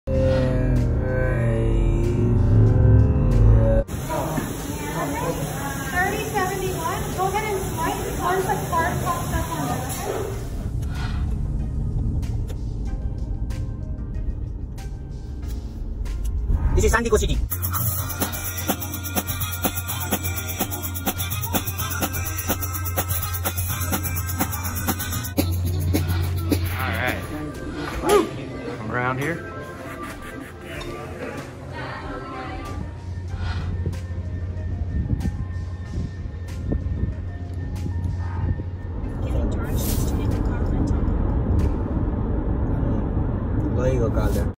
Travel New York To California Day 08
We stopped few hours at San Diego city and we were heading Los Angeles.